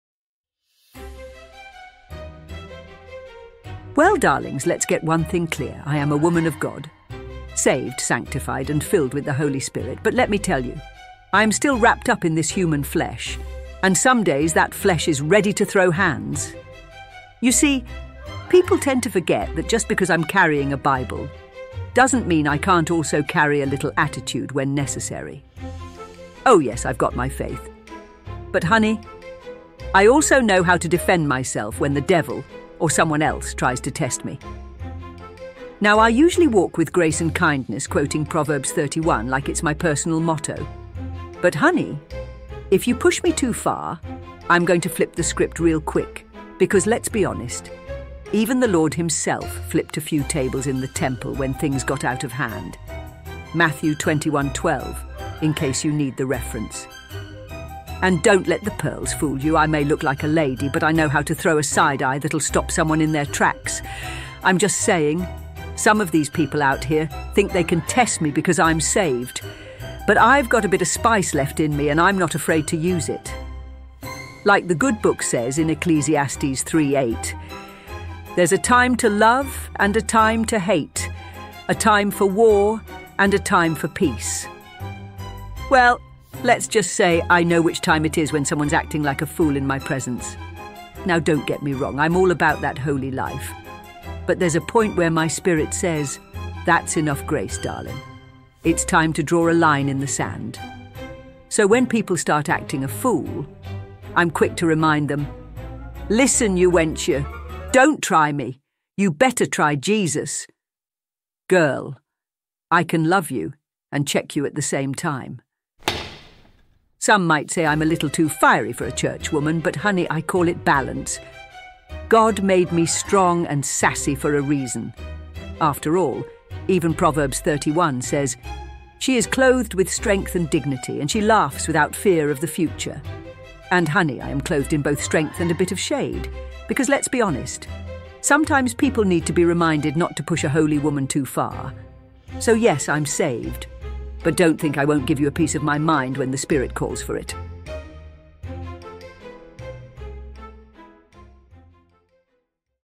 "I'm a Woman of God, But Don't Push Me"
Lady Landa is back with her fiery spirit and holy grace in perfect balance! In this hilarious and sassy tale, Lady Landa reminds us that while she’s saved, sanctified, and a true woman of God, she’s also not afraid to check anyone who crosses the line. With her Bible in one hand and a little attitude in the other, she delivers a funny and relatable story about holding her tongue (most of the time!) and standing her ground when necessary. After all, even a woman of faith has her limits—so don’t push her! This laugh-out-loud episode is all about finding that divine balance between grace and grit.